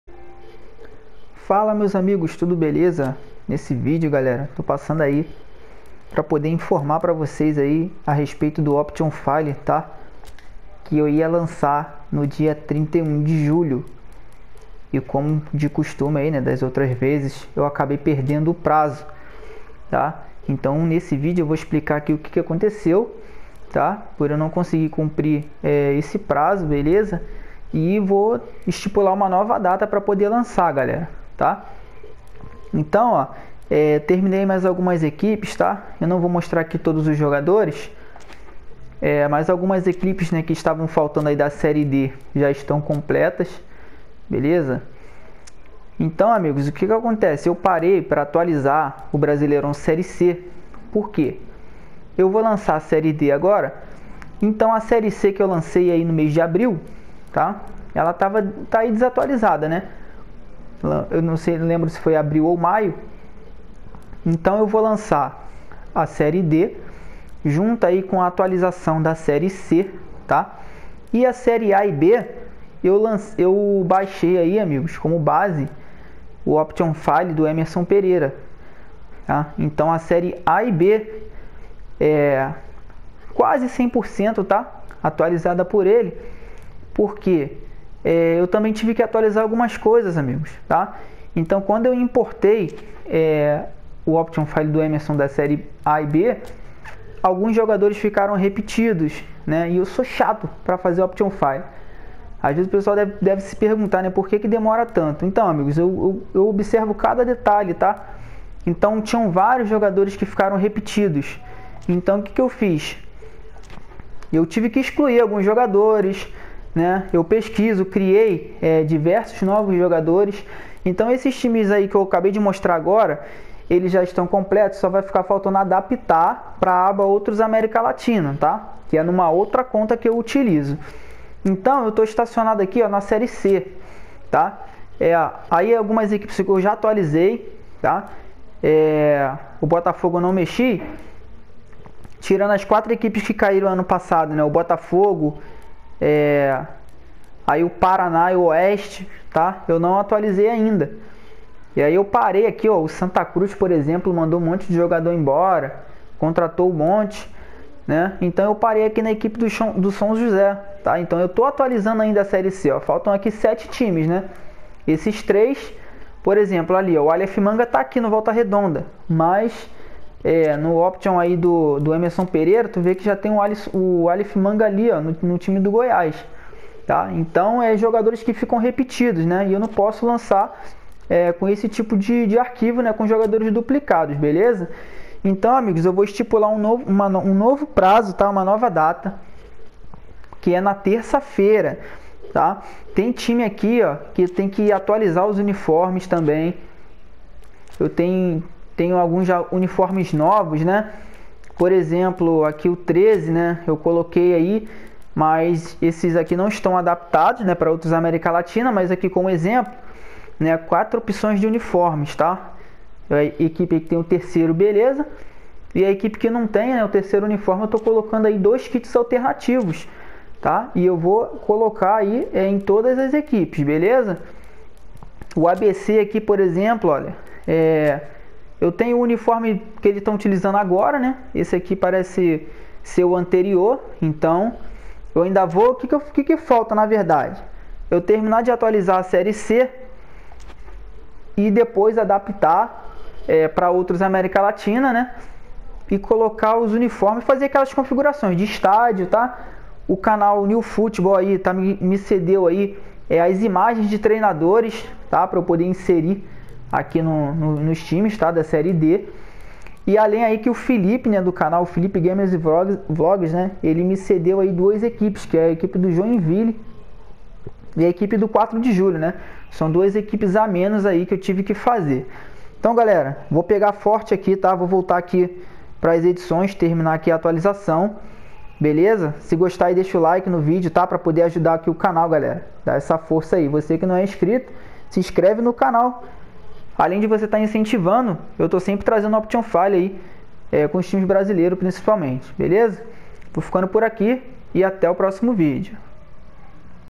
Option file serie D quase completo - ainda não disponível
Neste video eu explico o motivo do atraso do patch